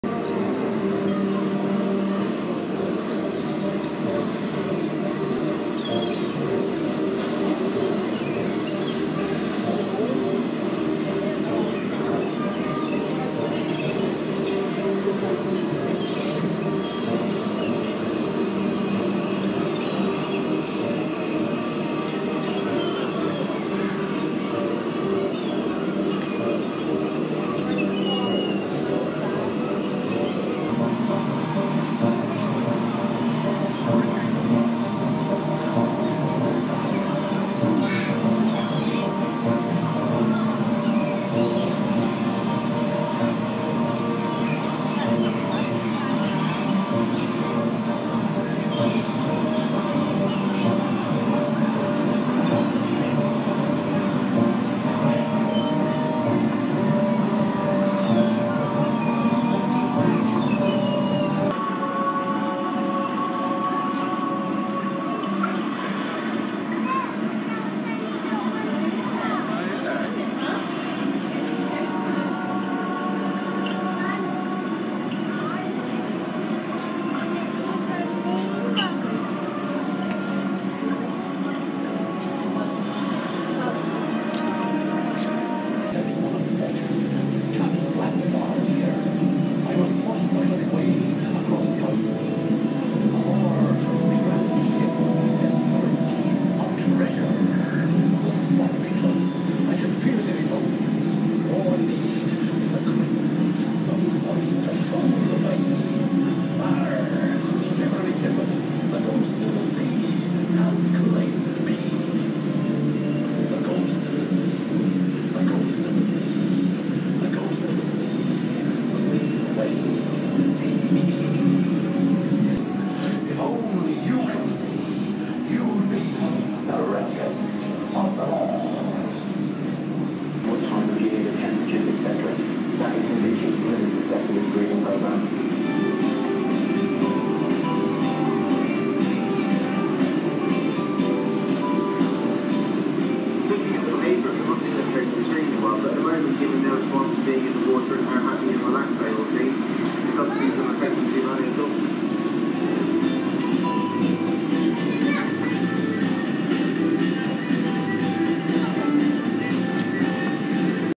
Sharkbait reef by sea life at Alton Towers 2016
Mum,Dad and I went to Sharkbait Reef by sea life at Alton Towers 2016 and we saw lots of fishes and it was great!